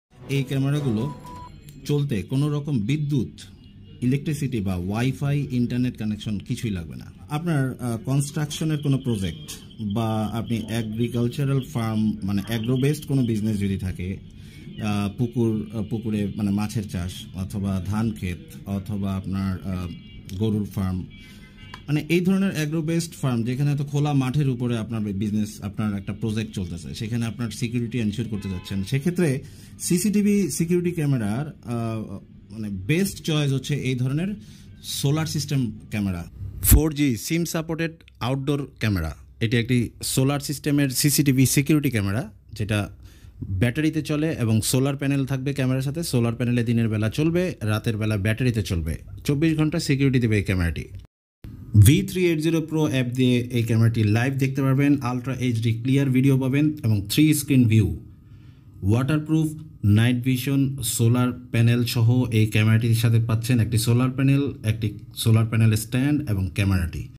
Best 4G Solar Double Lens Surveillance CCTV Camera In BD || V380 Pro 4G Camera
Best dual lens 4G cctv solar outdoor waterproof security camera in Bangladesh now. 4G solar camera ensure your security 24/7